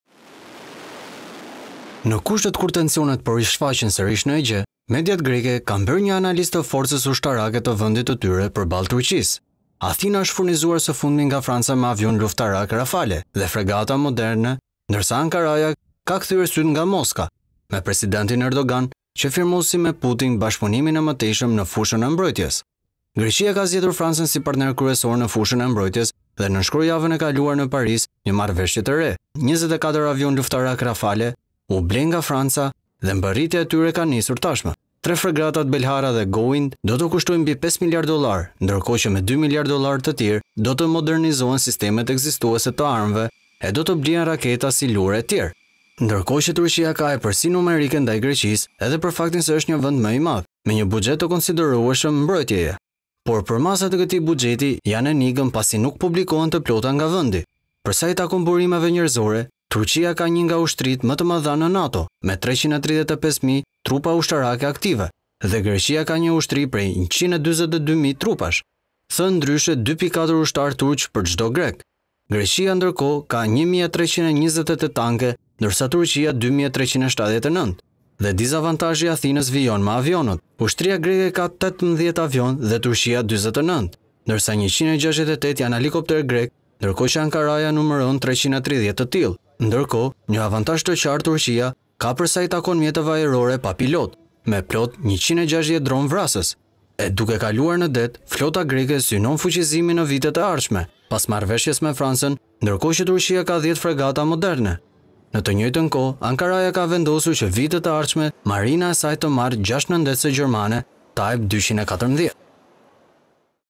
Top News - Ushtria greke apo Turke / Kush është më e fortë? Mediat nxjerrin shifrat
Në kushtet kur tensionet po rishfaqen sërish në Egje, mediat greke, kanë bërë një analizë të forcës ushtarake të vendit të tyre përballë Turqisë. Athina, është furnizuar së fundmi nga franca me avionë luftarakë Rafale dhe fregata të moderne ndërsa Ankaraja, ka kthyer sytë nga Moska, me presidentin Erdogan që firmosi me Putinin bashkëpunimin e mëtejshëm në fushën e mbrojtjes. Greqia ka zgjedhur Francën si partner kryesorë në fushën e mbrojtjes dhe nënshkroi javën e kaluar në paris një marrëveshje të re.24 avionë luftarakë Rafale u blenë nga franca dhe mbërritja e tyre ka nisur tashmë. Tre fregatat Belharra dhe Goëind, do të kushtojnë mbi 5 miliardë dollarë ndërkohë që me 2 miliardë dollarë të tjerë do të modernizohen sistemet ekzistuese të armëve e do të blihen raketa, silurë etj. Ndërkohë që turqia ka epërsi numerike ndaj Greqisë edhe për faktin se ëhstë një vend më i madh, me një buxhet të konsiderueshëm mbrojtjeje. Por përmasat e këtij buxheti, janë enigmë pasi nuk publikohen të plota nga vendi. Për sa i takon burimeve njerëzore, turqia ka një nga ushtritë më të mëdha në NATO me 355 mijë trupa ushtarake aktive dhe greqia ka një ushtri prej 142 mijë trupash. Thënë ndryshe, 2.4 ushtarë turq për cdo grek. Greqia ndërkohë ka 1328 tanke ndërsa turqia 2379 dhe disavantazhi i athinës vijon me avionët. Ushtria greke ka 18 avionë dhe turqia 49 ndërsa 168 janë helikopterët grekë ndërkohë që ankaraja numëron 330 të tillë. Ndërkohë, një avantazh të qartë Turqia ka për sa i takon mjeteve ajrore pa pilotë, me plot 160 dronë vrasës. E duke kaluar në det, Flota greke synon fuqizimin në vitet e ardhshme, pas marrëveshjeve me francën ndërkohë që Turqia ka 10 fregata moderne. Në të njëjtën kohë, Ankaraja ka vendosur që vitet e ardhshme që Marina e saj të marrë 6 nëndetëse gjermane “Type 214”.

Source:AFP